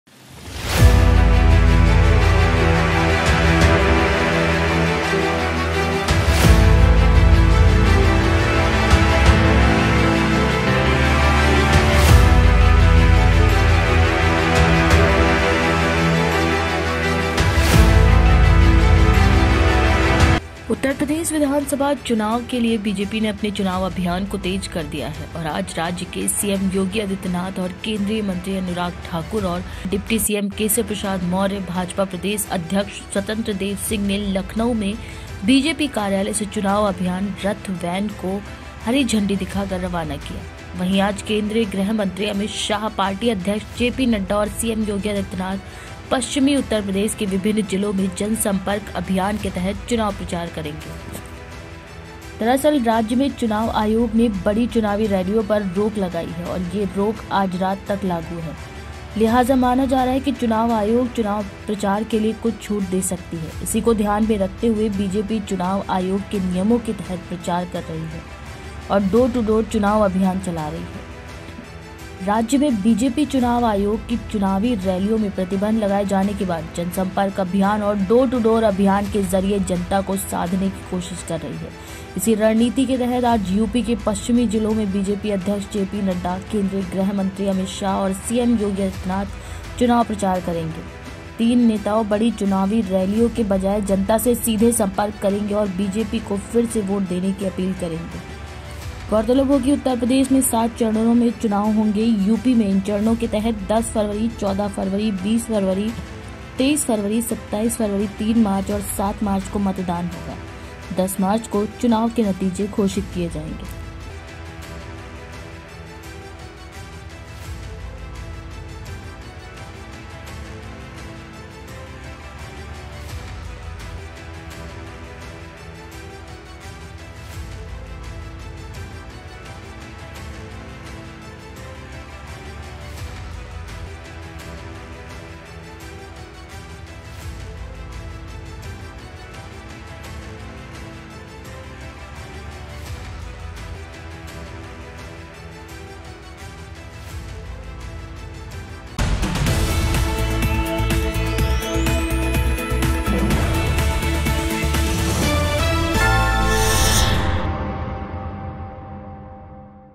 यूपी चुनावी अभियान को किया तेज, सीएम योगी ने चुनावी रथ को दिखाई हरी झंडी
liveindianews.org यूपी चुनावी अभियान को किया तेज, सीएम योगी ने चुनावी रथ को दिखाई हरी झंडीउत्तर प्रदेश विधानसभा चुनाव के लिए बीजेपी ने अपने चुनाव अभियान को तेज कर दिया है और आज राज्य के सीएम योगी आदित्यनाथ और केंद्रीय मंत्री अनुराग ठाकुर और मुख्यमंत्री योगी आदित्यनाथ, डिप्टी सीएम केशव प्रसाद मौर्य, भाजपा प्रदेश अध्यक्ष स्वतंत्र देव सिंह ने लखनऊ में बीजेपी कार्यालय से चुनाव अभियान “रथ” (वैन) को हरी झंडी दिखाकर रवाना किया। वही आज केंद्रीय गृह मंत्री अमित शाह पार्टी अध्यक्ष जेपी नड्डा और सीएम योगी आदित्यनाथ पश्चिमी उत्तर प्रदेश क विभिन्न जिलों में जनसंपर्क अभियान के तहत चुनाव प्रचार करेंगे।